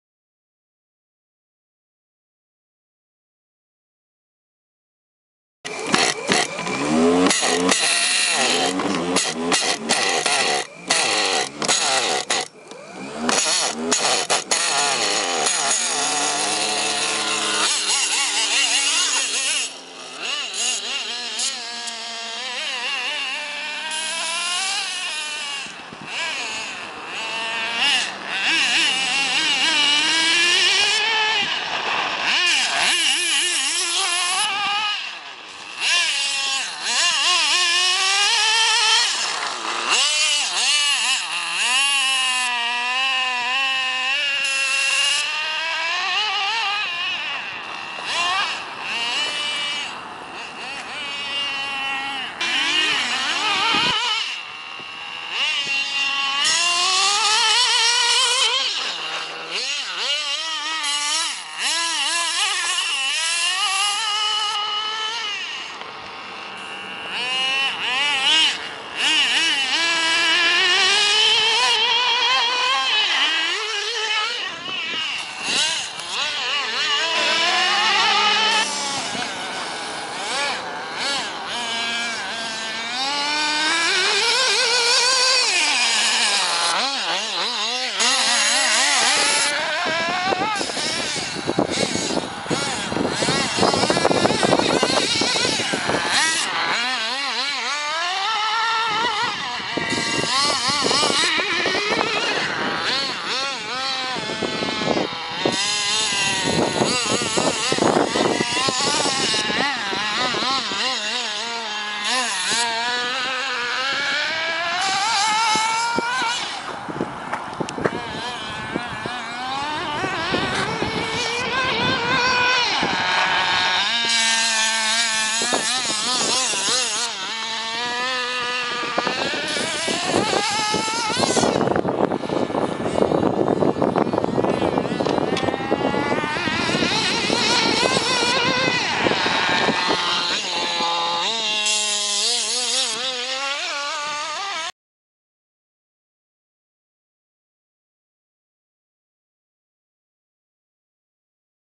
Mantua garbo mitika Vintage race buggy 1992
After a trade I have a first run with this vintage Garbo Mitika offroad buggy.
And the chippola .21 buggy engine is running very well for it's age.
Cheers,Alex